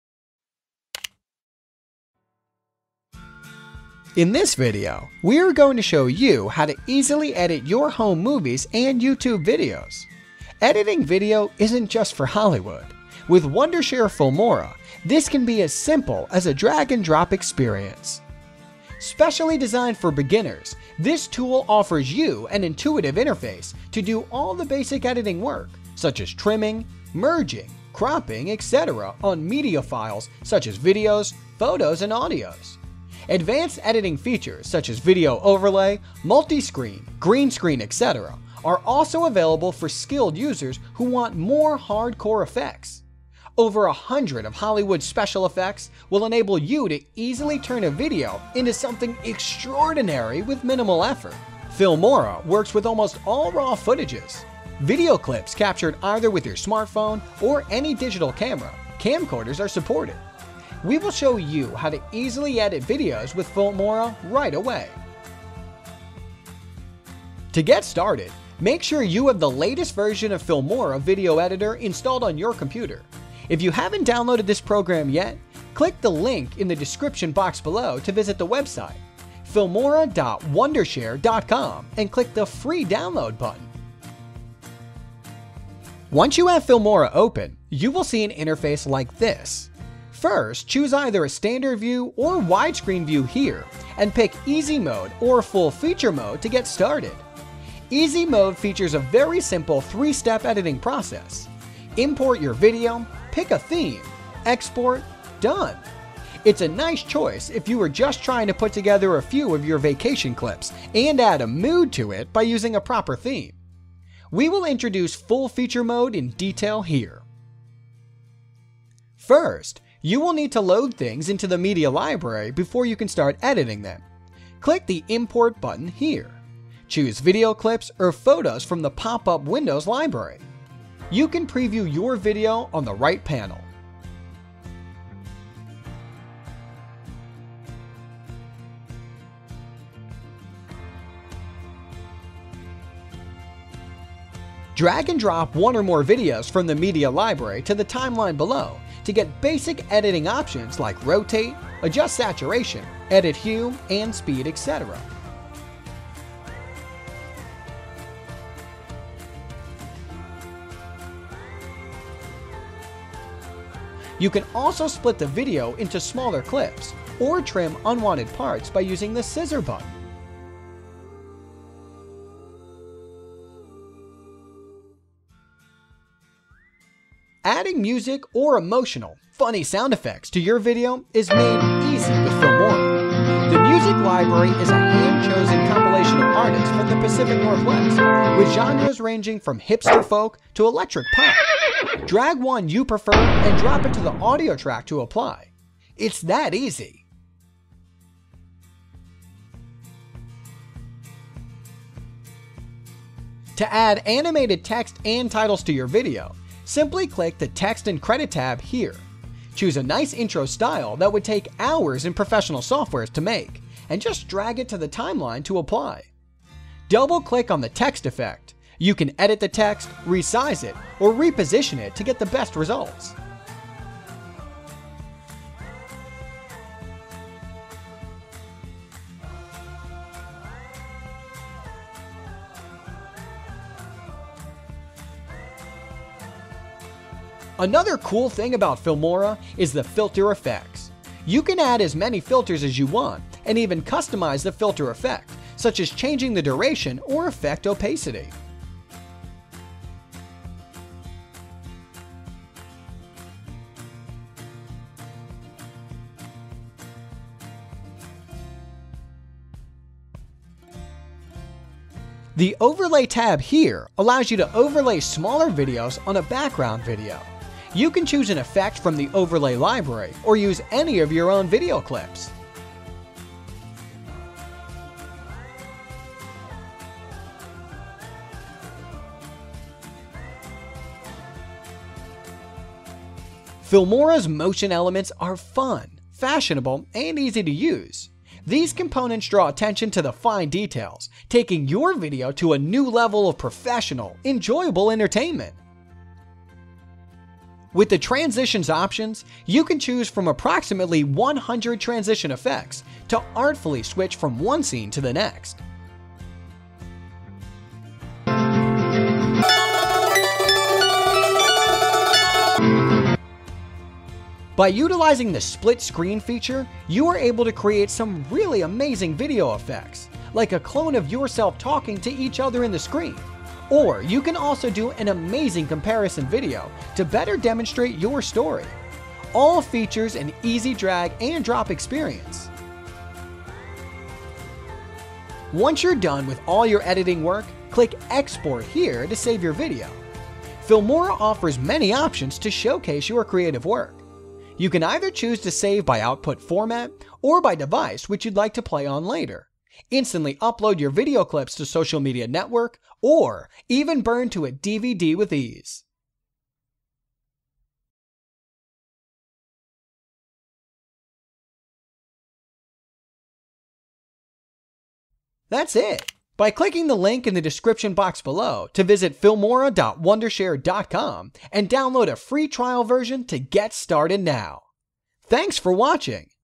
How to edit home movies and Youtube videos: Tutorial
In this video, we are going to show you how to easily edit your home movies and Youtube videos.  Editing video isn’t just for Hollywood. With Wondershare Filmora, this can be as simple as a drag and drop experience.